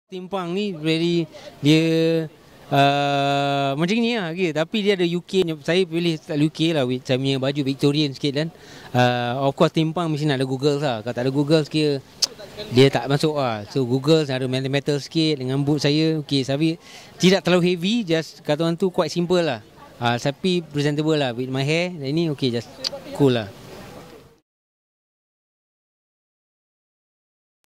Over The Top Amy!
Just love the "over-the-top" punk rock fashion style of Amy Search.  A true fashion blast is Amy, from head to toe! Amy Search was spotted by Trendsetter Online team during SHOUT! Award at Surf Beach, Sunway Lagoon.